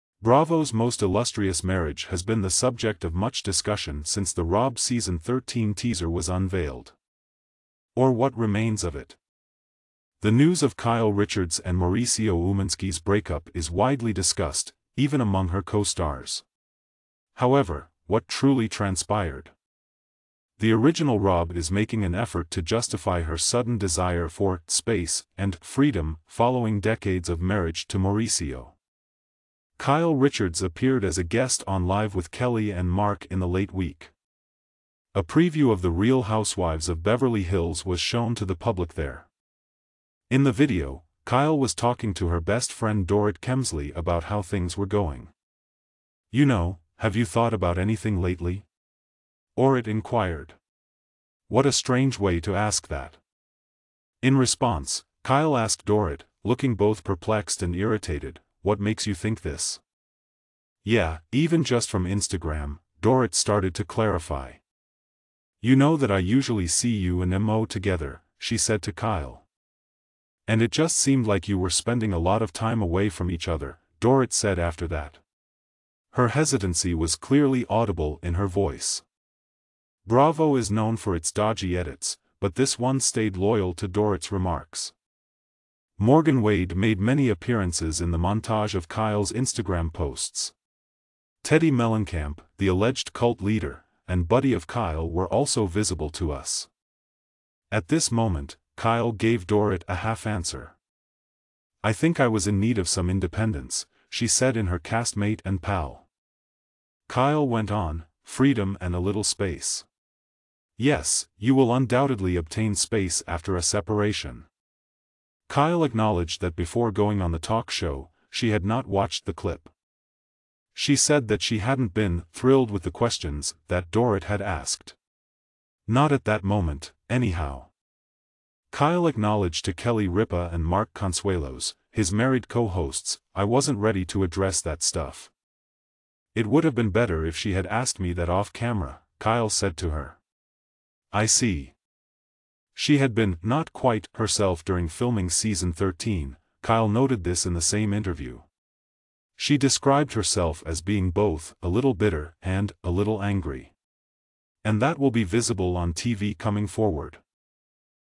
Kyle Richards: Mauricio Had to Give Me Space and Freedom!
Bravo's most illustrious marriage has been the subject of much discussion since the RHOBH Season 13 teaser was unveiled.
or what remains of it.
The news of Kyle Richards and Mauricio Umansky's breakup is widely discussed, even among her co-stars. However, what truly transpired.
The original RHOBH is making an effort to justify her sudden desire for "space" and "freedom" following decades of marriage to Mauricio.
Kyle Richards appeared as a guest on Live with Kelly and Mark in the late week.
A preview of The Real Housewives of Beverly Hills was shown to the public there.
In the video, Kyle was talking to her best friend Dorit Kemsley about how things were going.
"You know, have you thought about anything lately?" Orit enquired. What a strange way to ask that.

In response, Kyle asked Dorit, looking both perplexed and irritated, "What makes you think this?"
"Yeah, even just from Instagram," Dorit started to clarify.
"You know that I usually see you and Mo together," she said to Kyle.
"And it just seemed like you were spending a lot of time away from each other," Dorit said after that. Her hesitancy was clearly audible in her voice.
Bravo is known for its dodgy edits, but this one stayed loyal to Dorit's remarks.
Morgan Wade made many appearances in the montage of Kyle's Instagram posts. Teddi Mellencamp, the alleged cult leader, and buddy of Kyle were also visible to us.